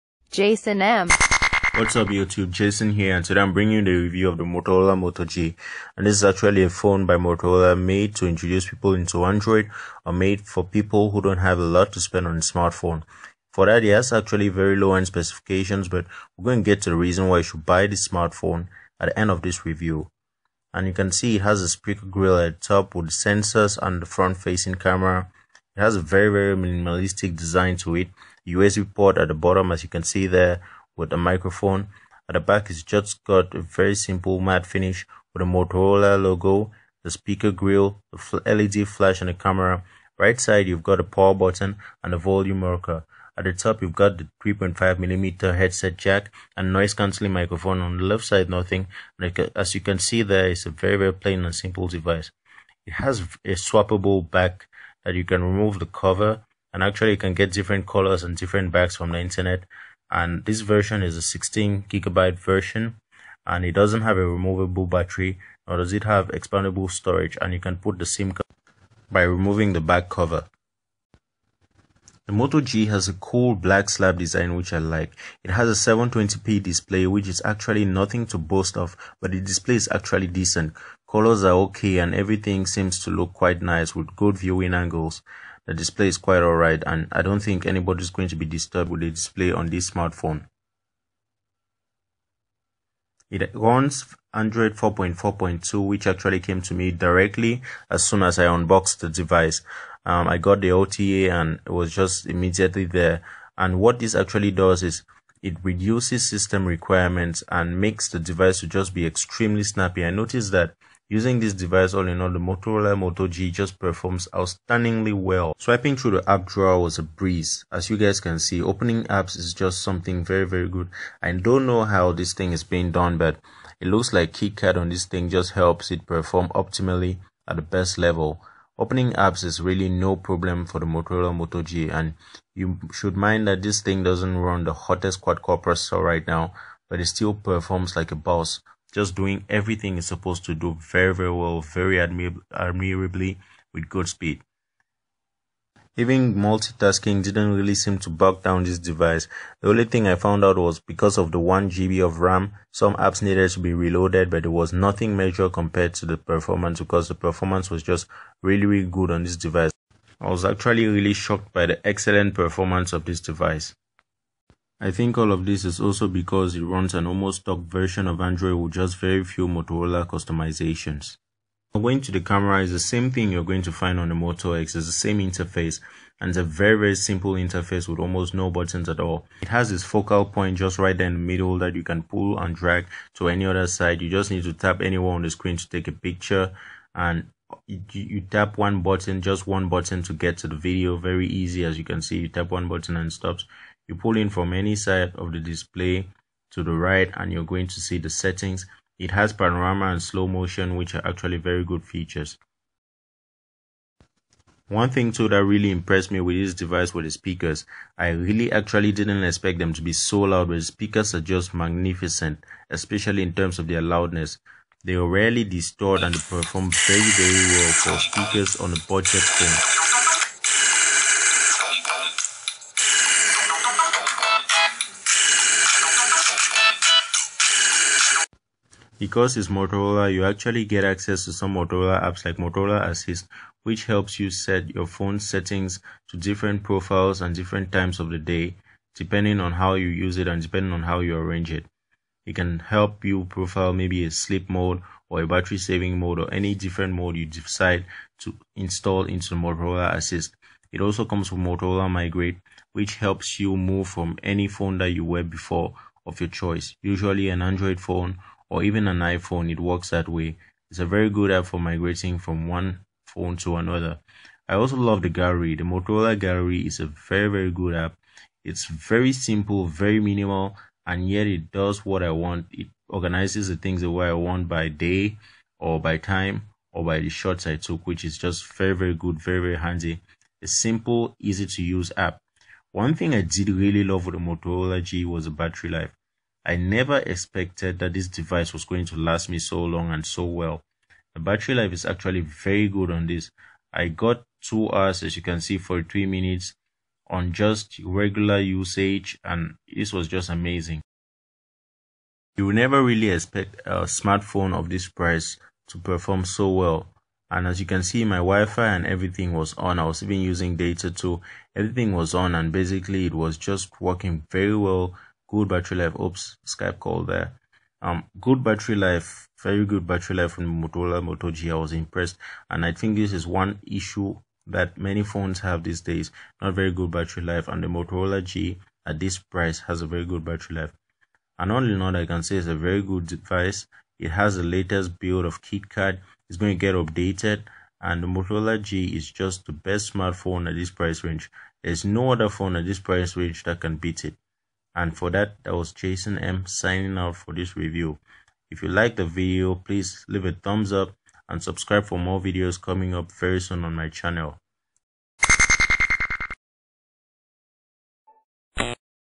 Moto G Review-Still Worth It Now?
My review of the Motorola Moto G.This is the 16GB version.See what I have to say about it.